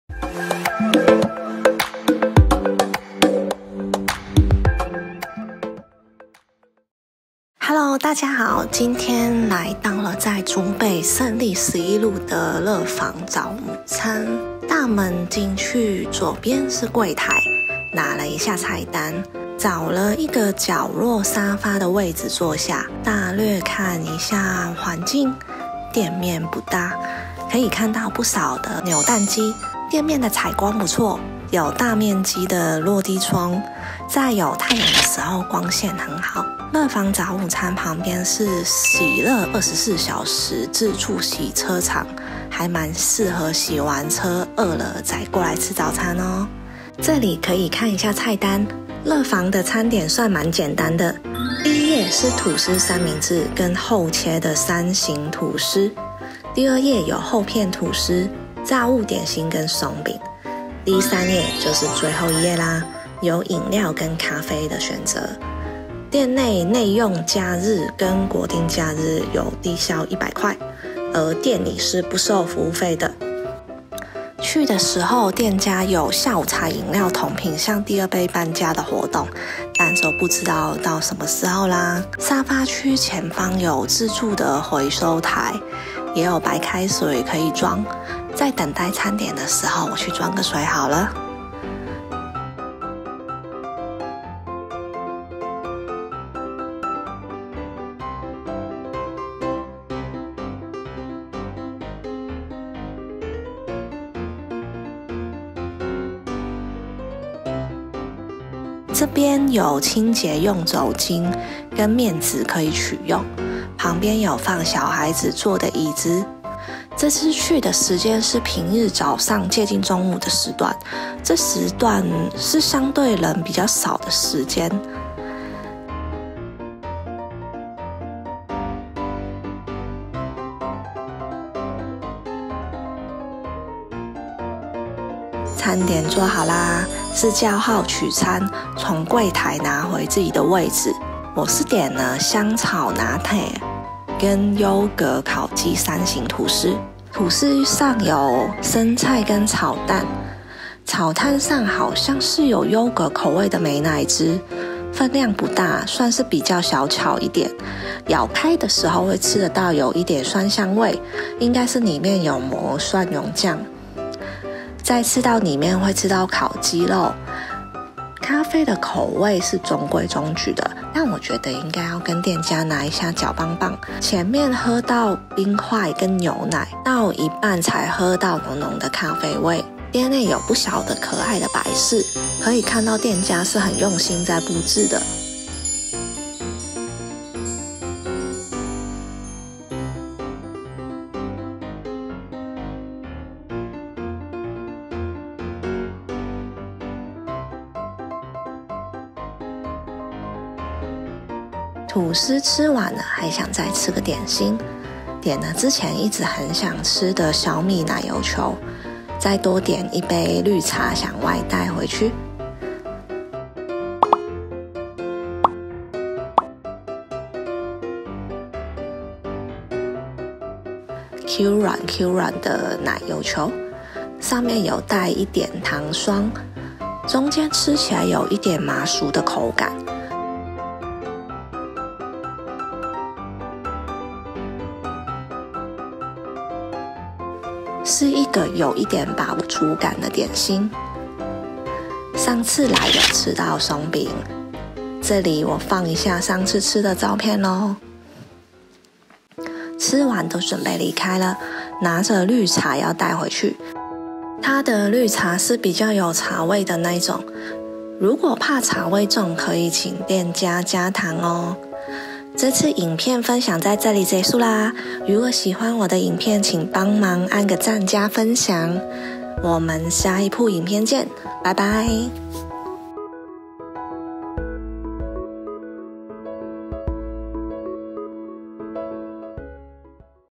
【The Fun 樂房早午餐】竹北The Fun 樂房早午餐。親子式早午餐店舒適環境適合家人情侶朋友一個人來享受早午餐時光2022
竹北樂房早午餐，環境舒適，好吃的吐司配飲料或咖啡～

餐廳地址：新竹縣竹北市勝利十一路230號
到訪時間：2022/11月底